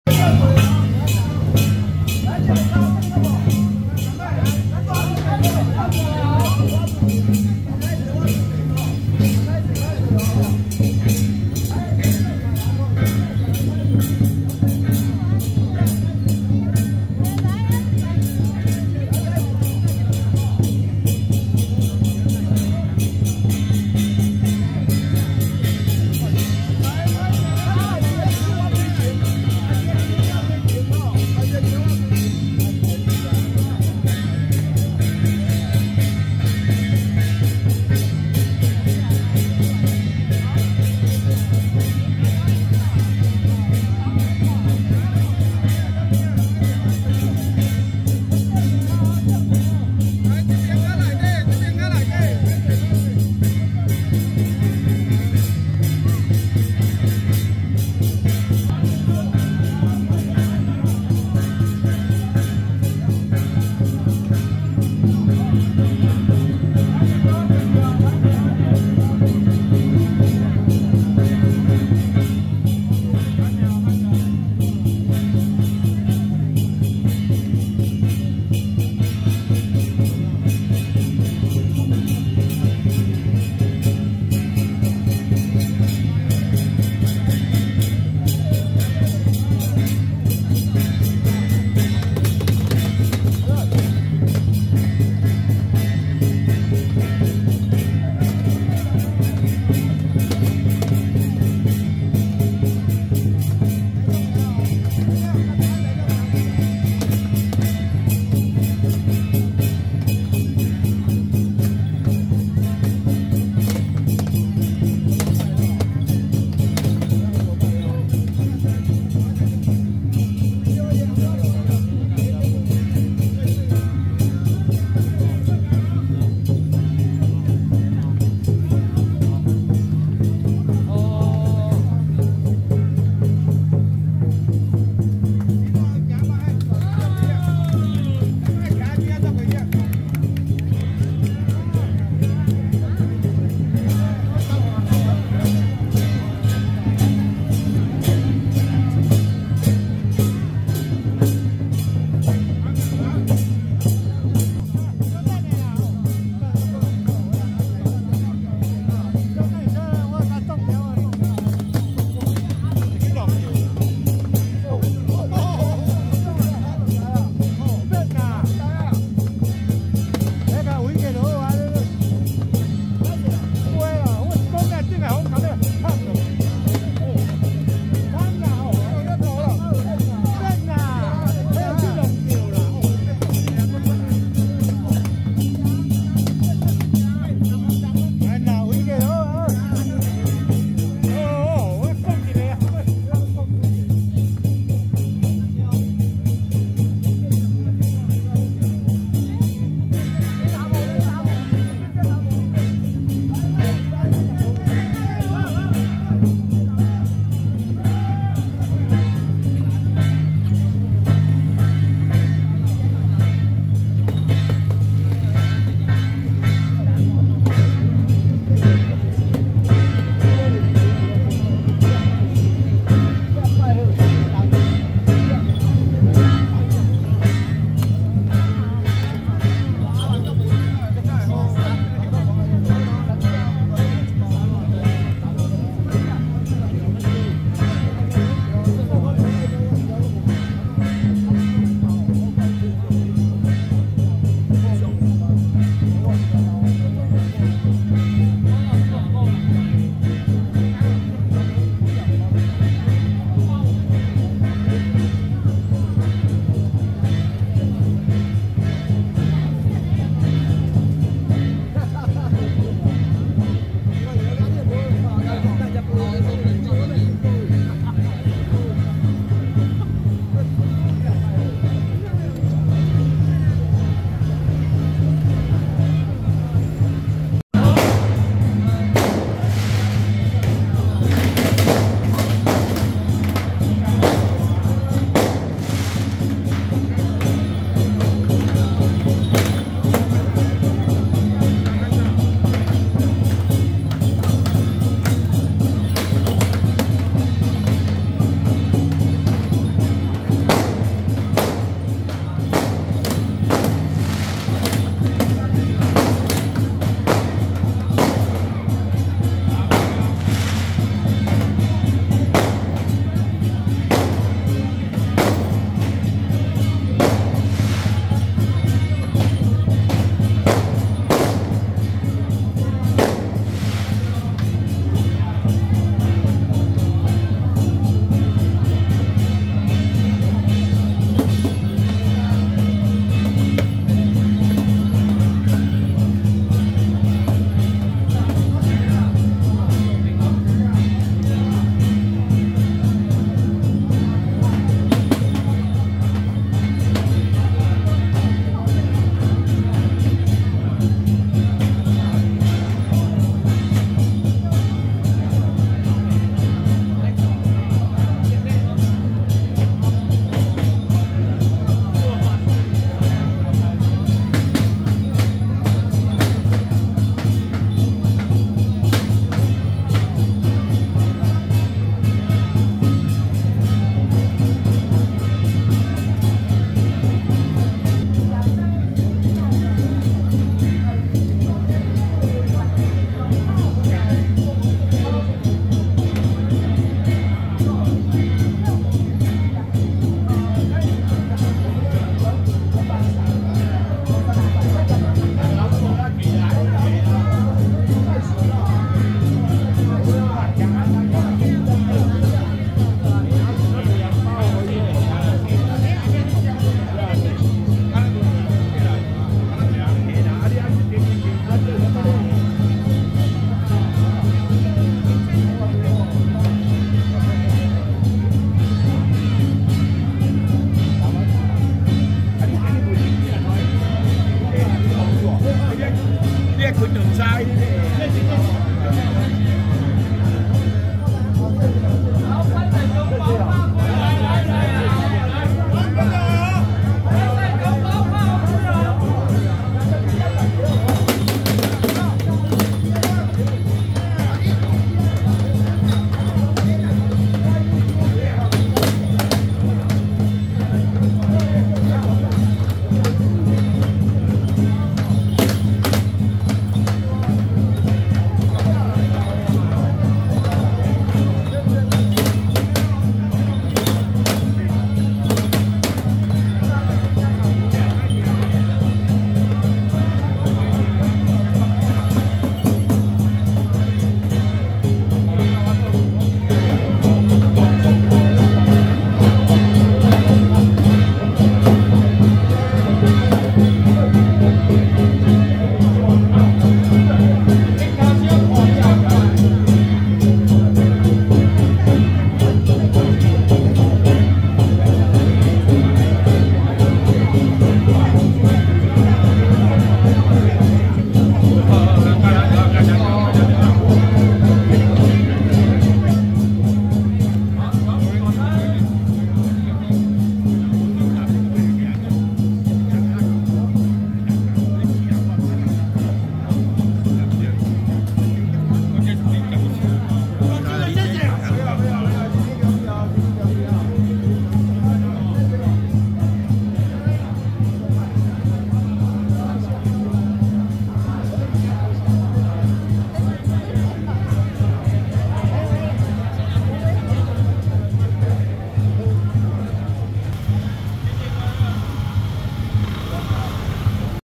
【過境 蘆洲湧蓮寺＆蘆洲文武大眾廟＆蘆洲保和宮 全記錄】~113蘆洲混龍獅行春踩街
蘆洲混龍獅-金獅團＆達摩祖師 神將(0:00)過境 蘆洲湧蓮寺 (2:28)過境 蘆洲文武大眾廟 (4:38)過境 蘆洲保和宮